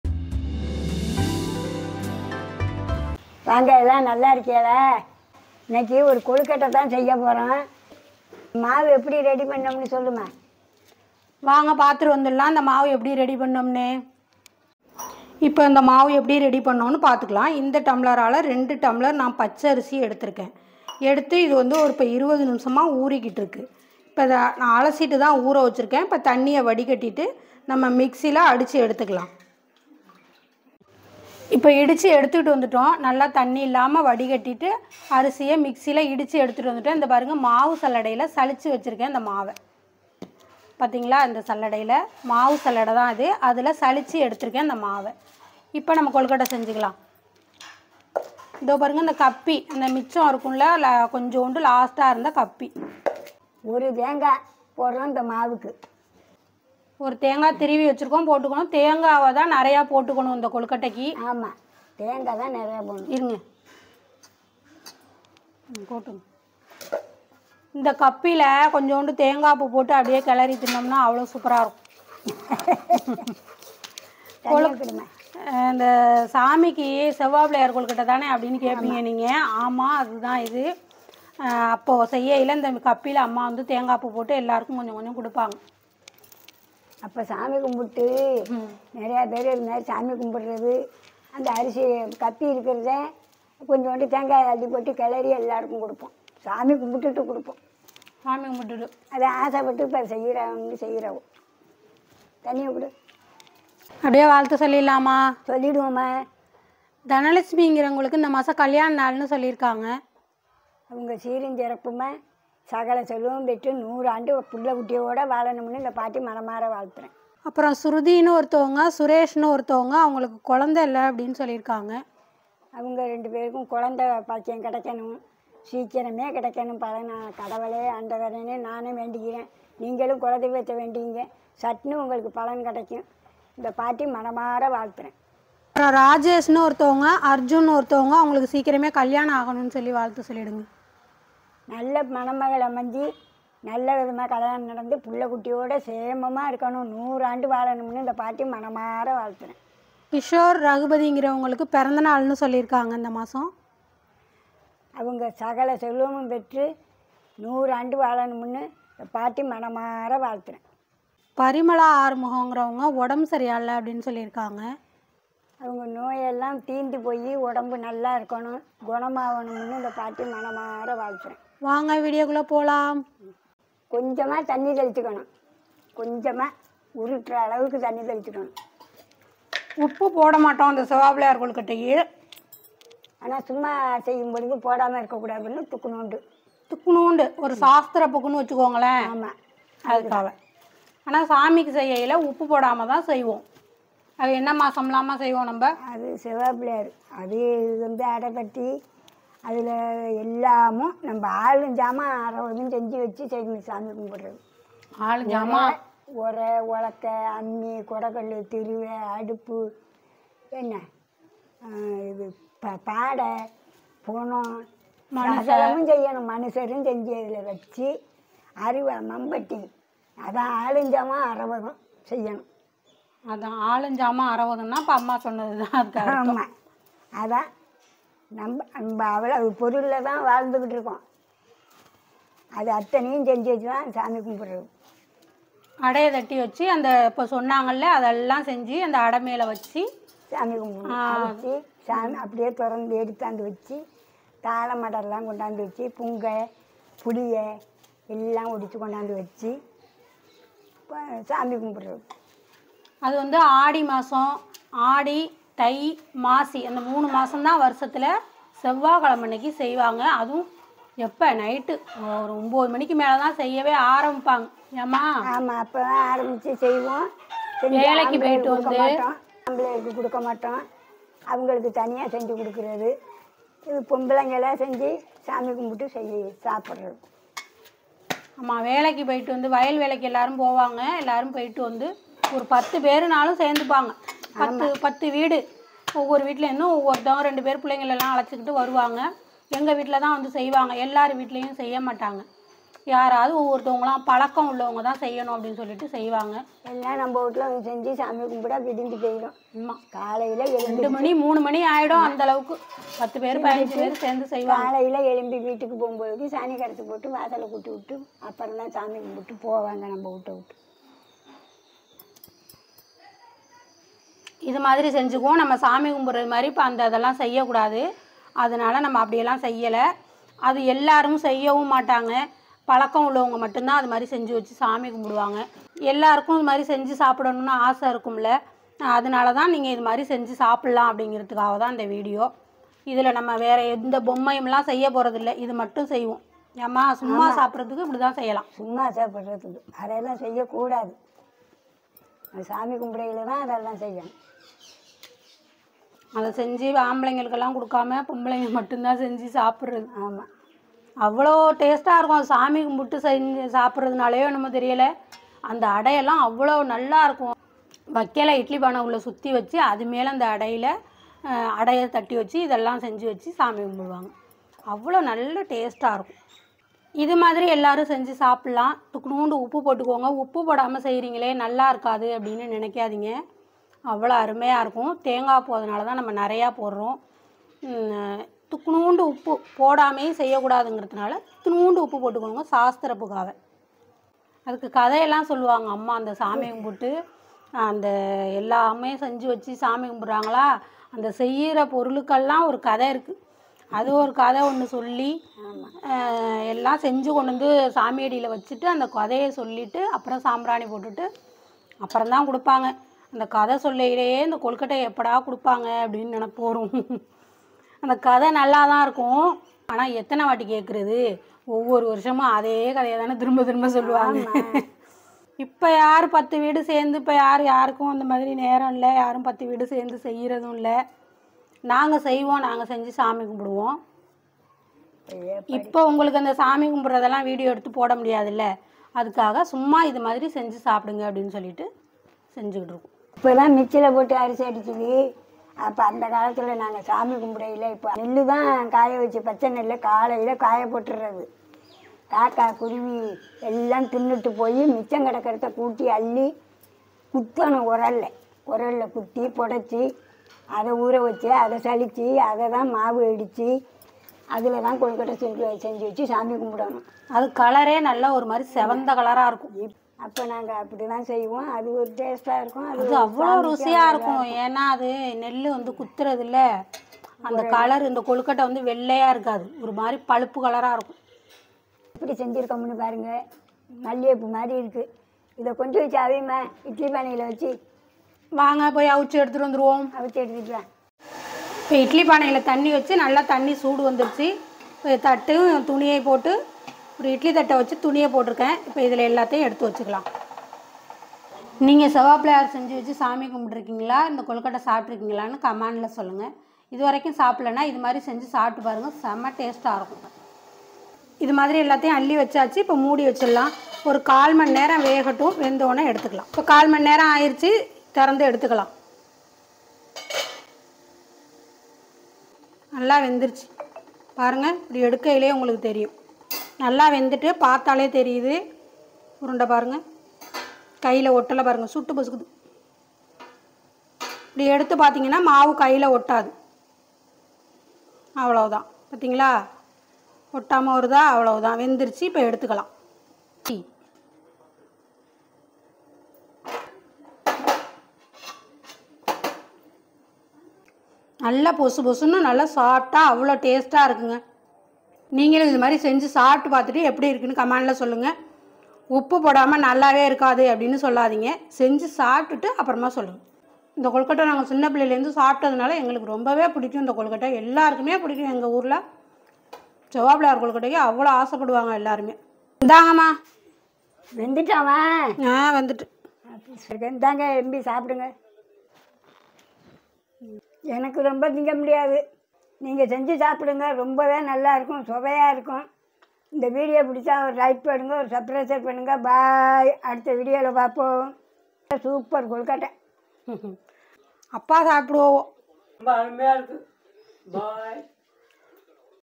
செவ்வாய் பிள்ளையார் கொழுக்கட்டை சாப்பிட்டுறிகிங்களா இது மாதிரி செஞ்சு சாப்பிடுங்க...
Siva pillayar kolukattai/Kolkata recipe in Tamil/snacks recipe in Tamil செவ்வாய் பிள்ளையார் கொழுக்கட்டை ரெசிபி